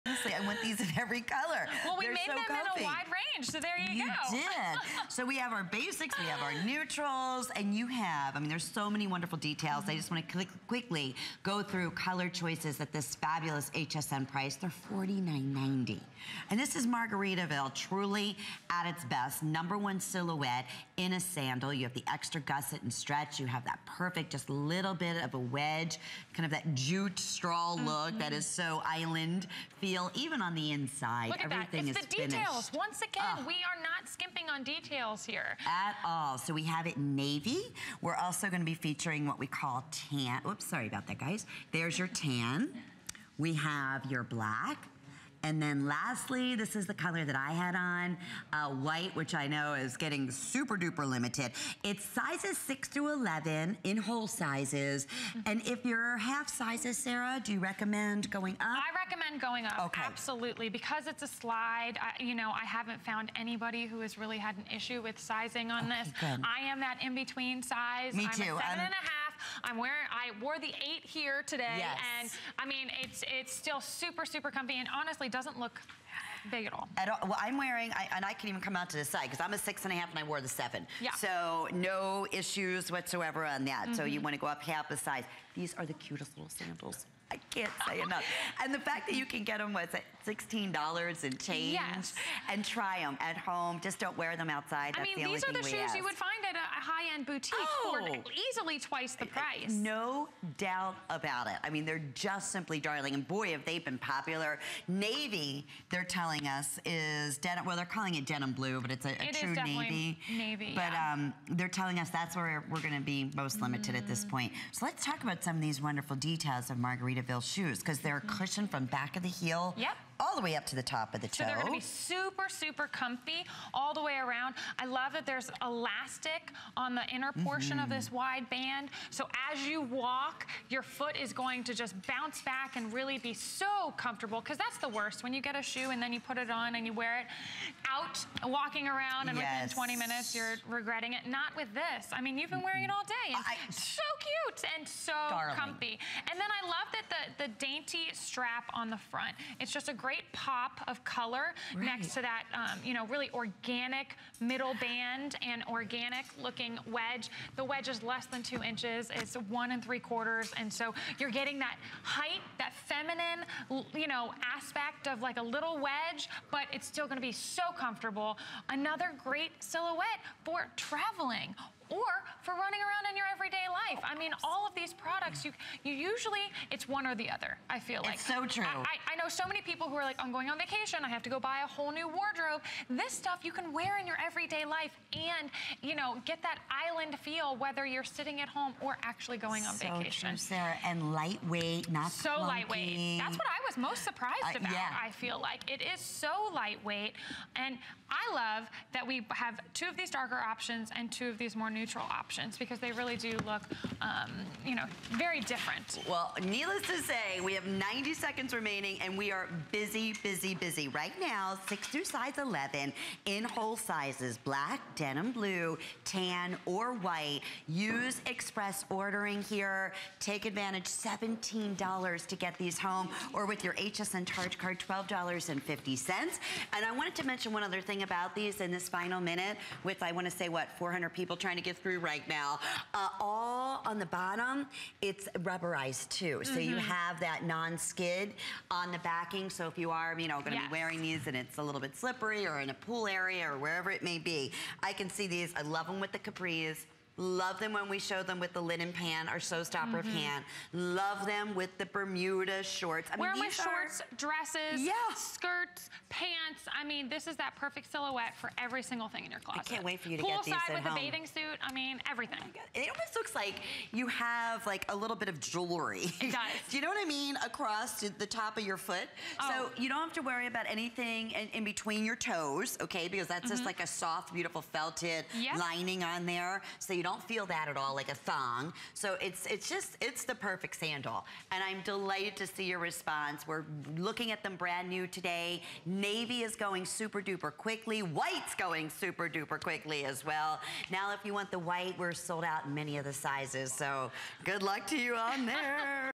Margaritaville Raffia Wedge ToeLoop Sandal
Sunny days and sandals go together like margaritas and Jimmy Buffet music. Make these moments more stylish with this beachy wedge sandal. The...